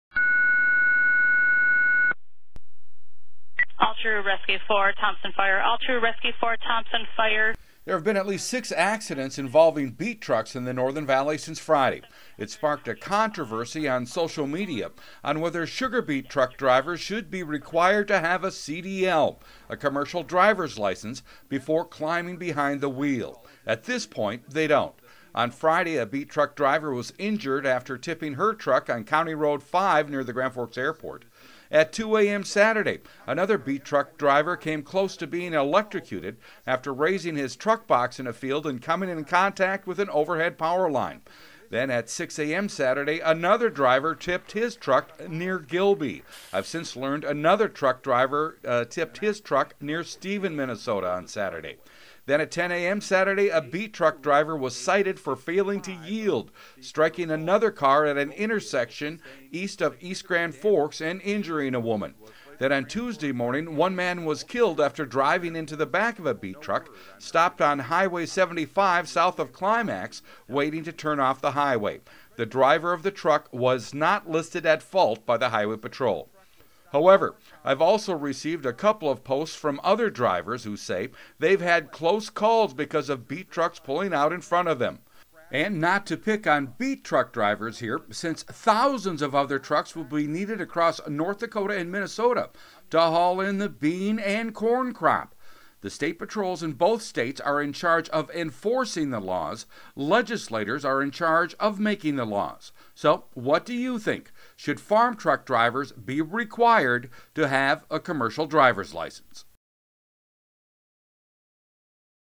Controversy Over Commercial Drivers Licenses Grows With Number Of Beet Truck Accidents
GRAND FORKS, ND (iNewZ.TV) The controversy is growing on social media on whether farm truck drivers should be required to have a commercial drivers licenses, as the number of beet truck accidents grows.